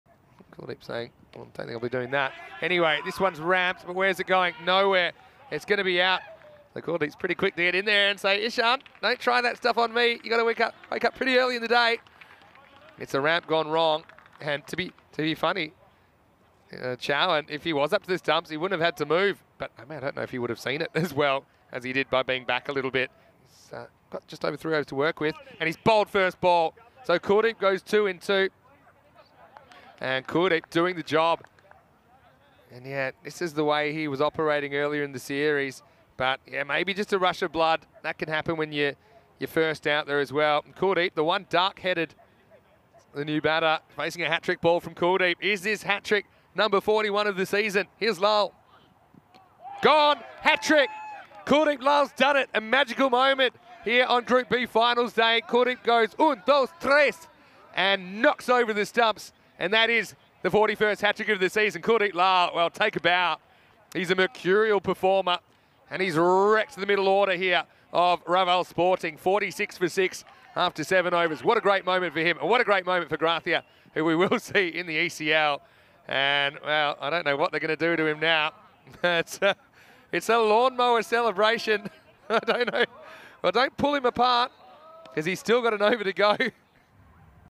POTM: K.Lal - Group B, Qualifier 1 GRA vs RAS Highlights FanCode ECS Spain, Barcelona 2022 ECS22.792
Player of the Match: Kulddep Lal (3/13) - Group B, Qualifier 1 - GRA vs RAS | Highlights | FanCode ECS Spain, Barcelona 2022 | ECS22.792